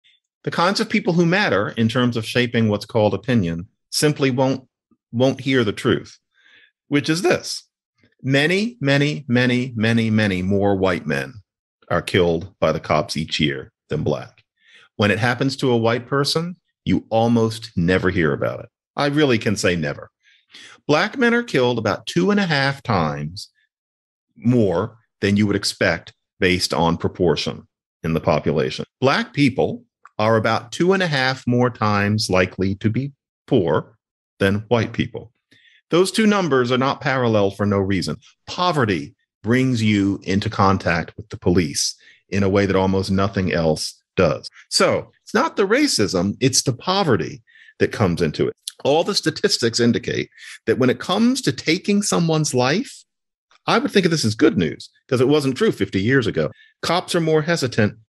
The truth about the cops and black America | John McWhorter
John McWhorter is a linguistics professor at Columbia University, an essayist for "The New York Times," and the author of "Woke Racism."